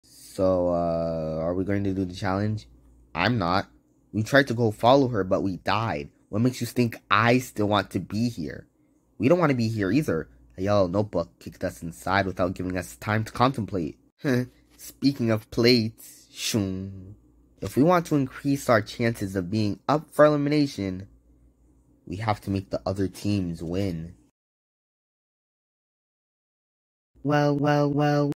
[CLIP] BFDIITRW2 IN TWO WEEKS
Burrito: are we going to do the challenge...
Battery: I'm not...we tried to go to follow her but we died.. what makes you think I still want to be here.
Bucket: we don't want to be here either...a yellow notebook kicked us inside without giving us time to contemplate.
Map: speaking of plates...
*plate of ice with the frozen players*
Map: If we want to increase our chances of being up for elimination, we have to make the other teams win...